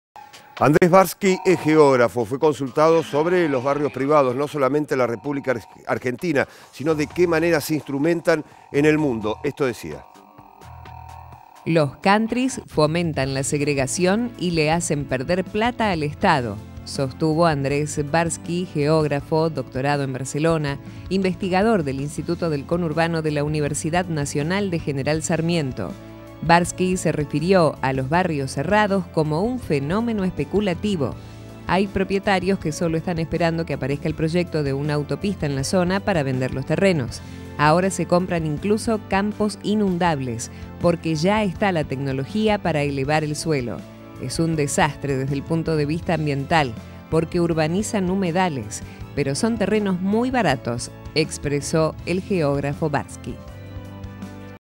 Más cuestionamientos al modelo de barrio cerrado
El geógrafo argentino Andres Barsky recordó que está cuestionado en “todo el mundo” y que “cristaliza la desigualdad”. Asegura que fomentan la segregación y le hacen perder dinero al Estado, además de ser un fenómeno especulativo.